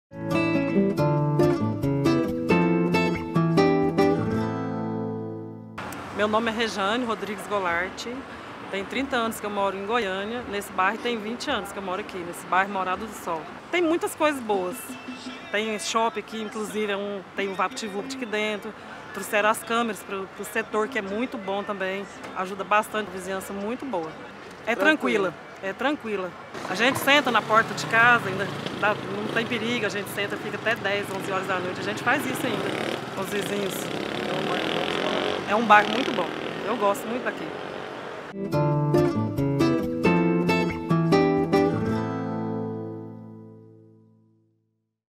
Goiânia em Tom Maior | Revele sua Cidade com Rejane Rodrigues
Em comemoração aos 86 anos de Goiânia, o Sistema Sagres foi as ruas da capital para ouvir  depoimentos daqueles que nasceram aqui e dos que fizem da cidade sua casa. Venha com a gente e revele a sua cidade!